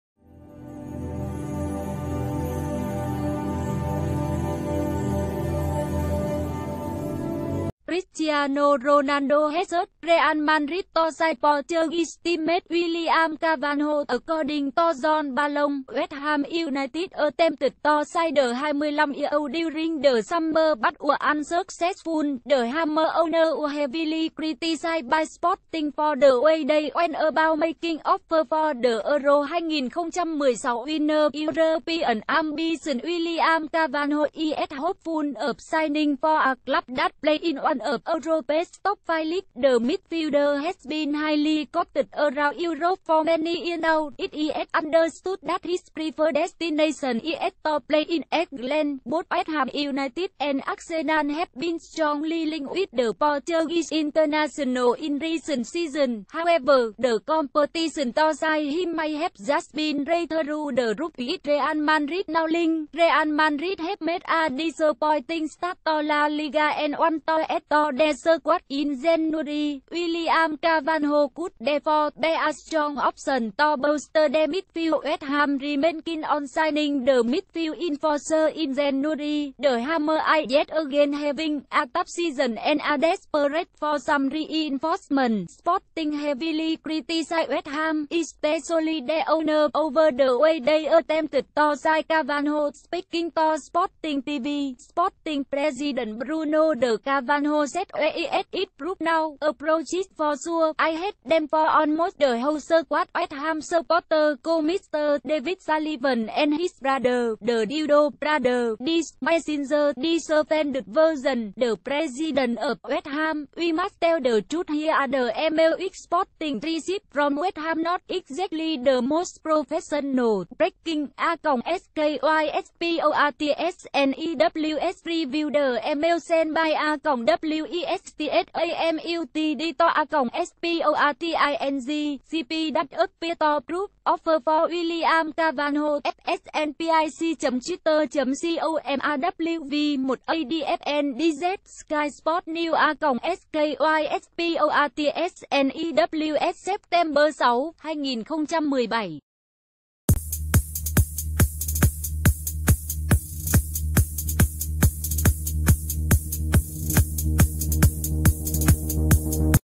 [NVC] Cristiano ronaldo urges real madrid to sign west ham target
Cristiano ronaldo urges real madrid to sign west ham target